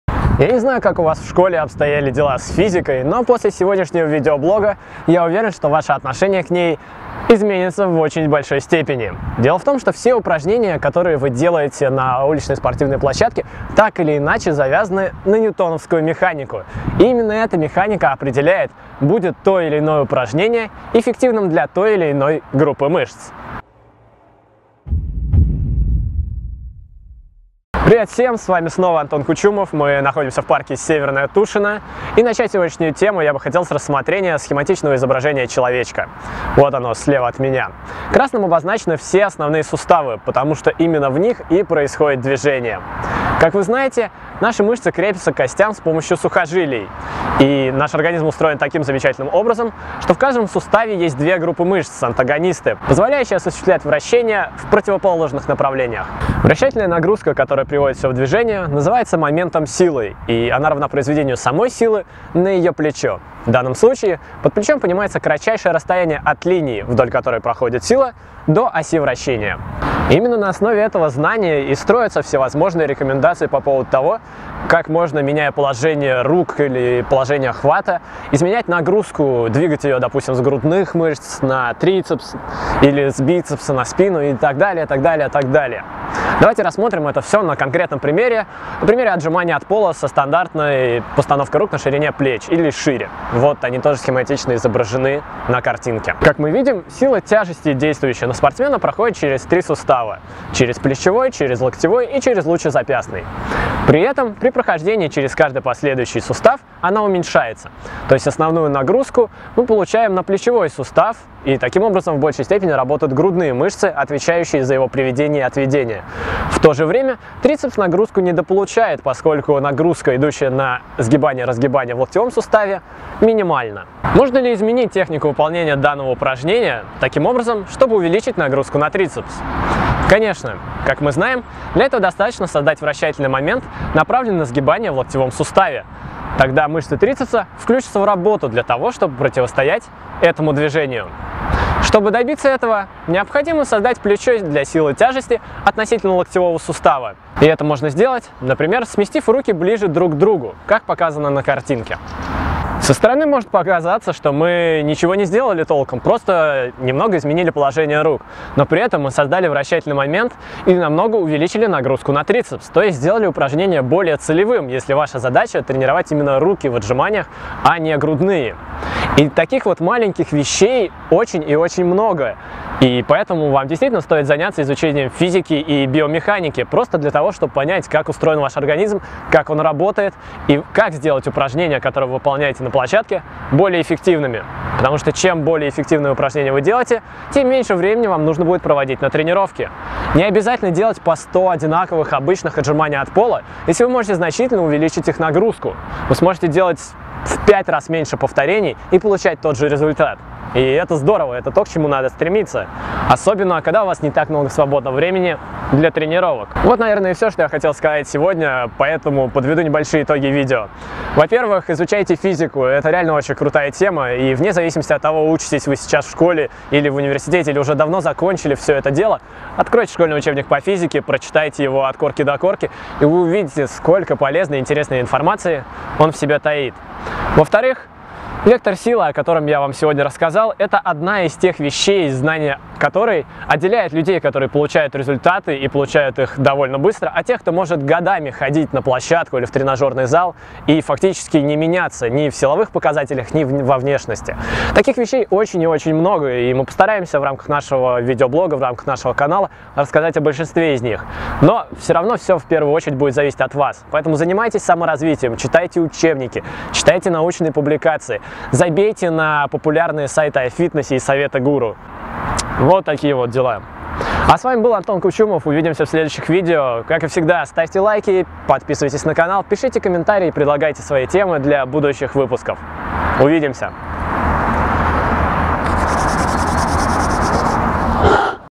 Момент силы | Антон Кучумов | 100-дневный воркаут - День 55
Как выбрать лучшее упражнение для той или иной группы мышц?  Каким образом связаны мышцы антагонисты и момент силы?  И почему знание физики и ньютоновской механике позволяют выполнять упражнения более эффективно?

◄ СОДЕРЖАНИЕ ►

00:35 Основные суставы в организме
00:55 Мышцы антагонисты
01:05 Момент силы
01:35 Работа силы тяжести на примере в отжиманий от пола
02:25 Как включить трицепс в отжиманиях от пола

◄ РЕКОМЕНДУЕМ! ►